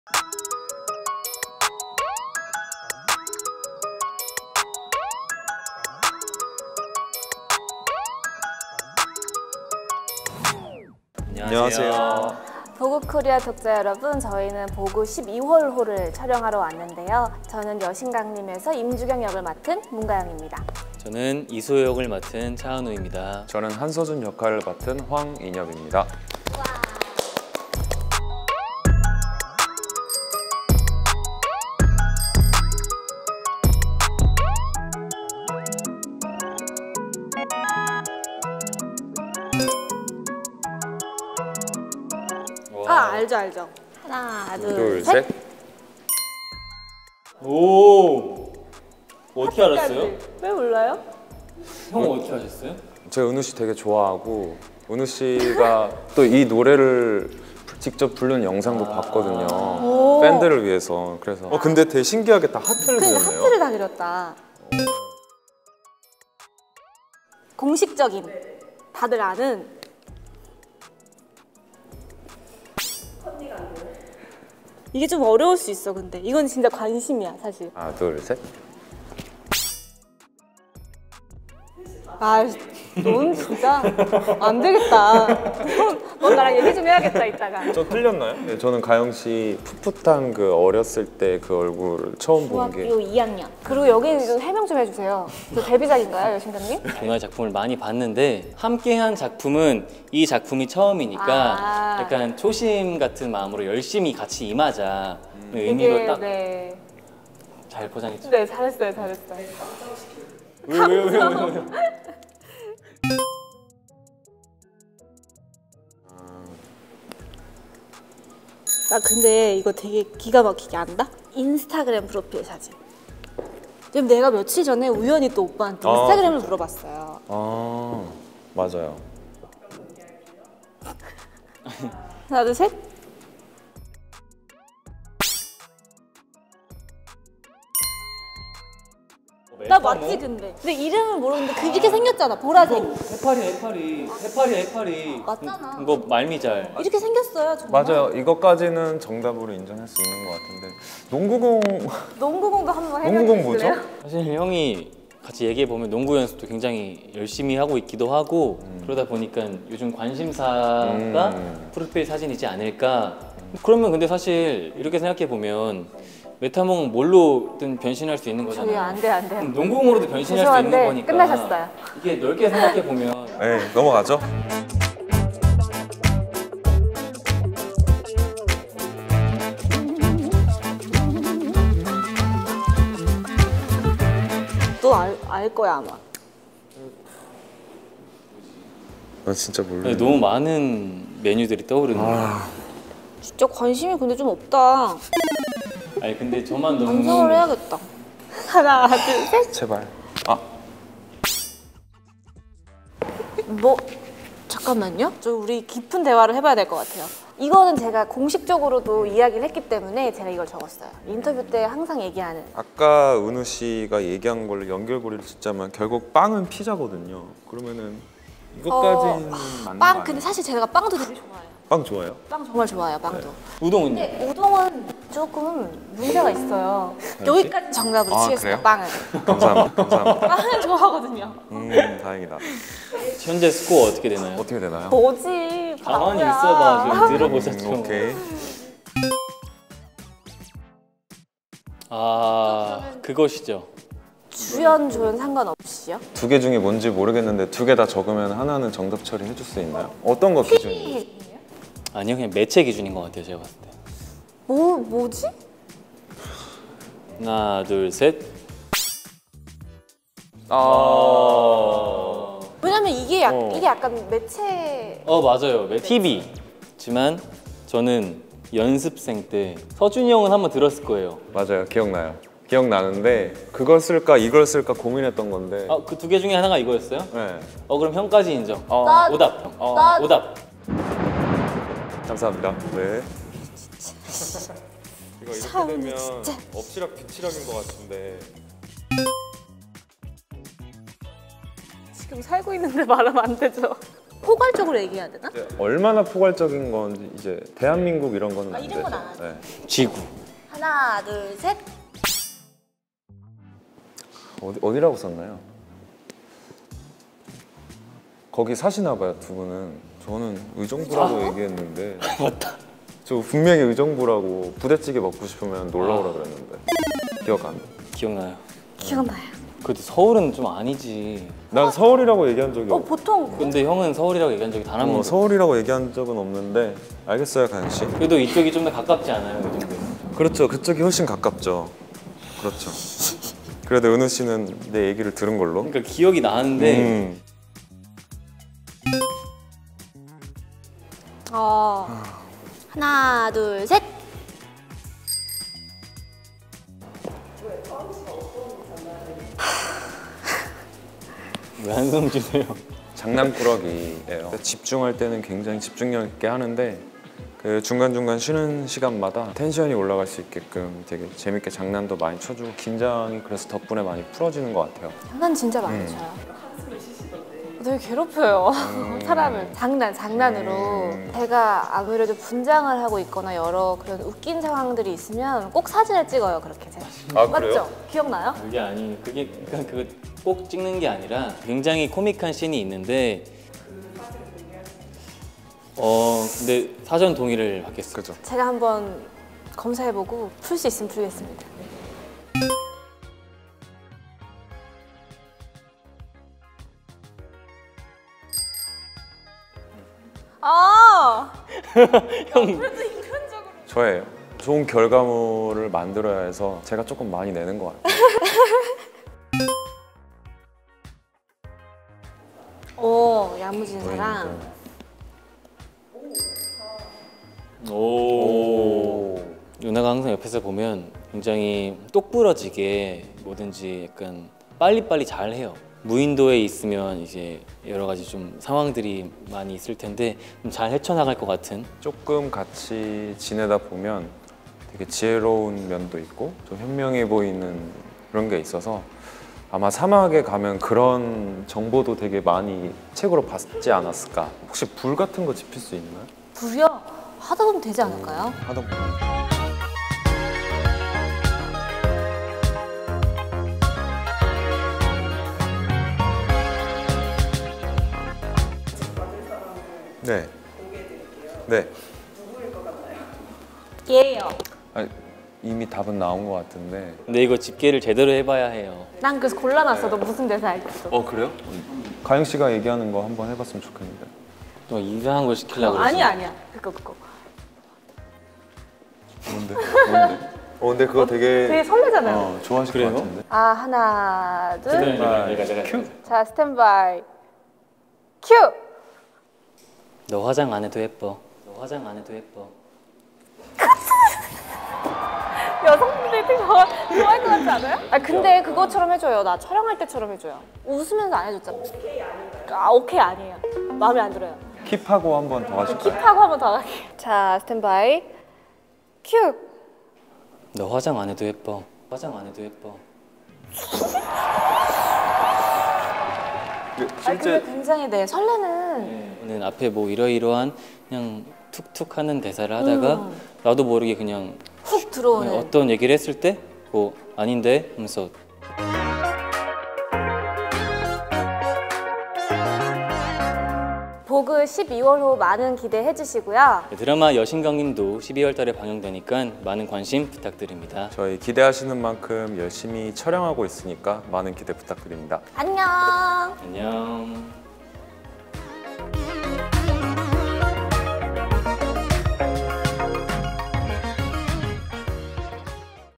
[여신강림] 싱크로율100% 역대급 캐스팅. 은근히 허당끼 있는 차은우?!ㅣ문가영,차은우,황인엽ㅣVOGUE MEETS
네이버의 인기 웹툰 [ 여신강림 ]이 드라마로 찾아왔습니다. 기존 캐릭터들과 싱크로율 100%의 배우들, 서로에 대해 얼마나 잘 알고 있을까요? 촬영장 비하인드부터 살짝 미리보기까지! 문가영, 차은우, 황인엽의 우정퀴즈를 지금 바로 확인하세요!
디지털에디터 ㅣ 강수지
디자인 ㅣ 장명식
촬영 ㅣ 주호연

*본 촬영은 모든 스태프가 소독 및 생활 속 거리두기 지침을 준수하며 진행했습니다*